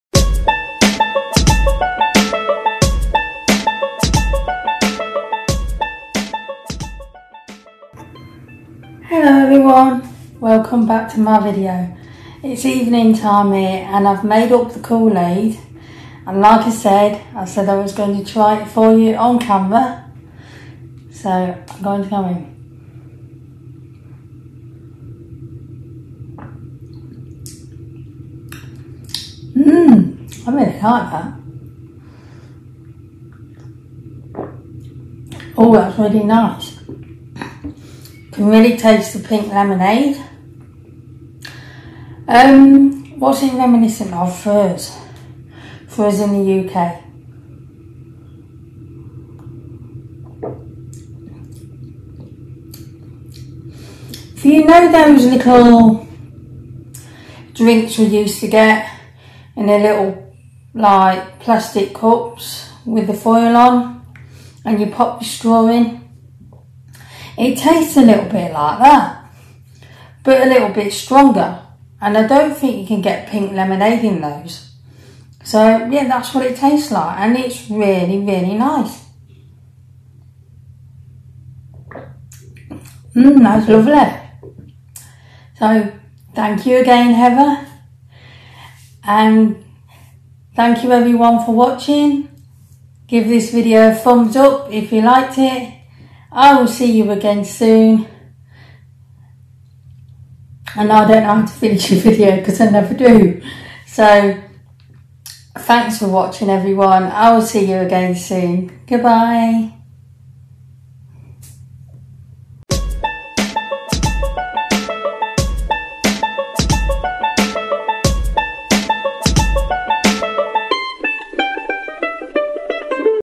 British Girl Tries Koolaid
I have a Bonus video for you today of me trying Koolaid for the first time.